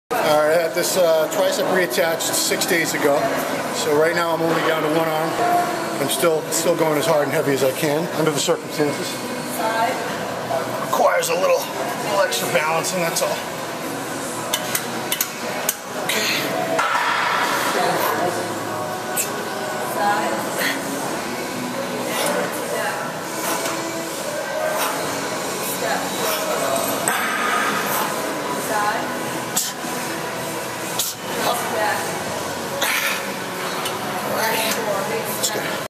1-arm standing db press 70 x 10
Down to one arm, but I have three weeks until that shoulder is operated on and I have to chill on ALL upper body training for a while.